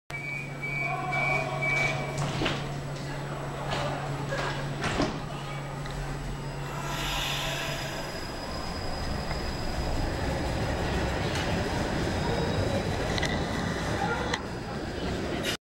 Antwerp subway tram folds mirror before entering the tunnel
Subway tram in Antwerp, Belgium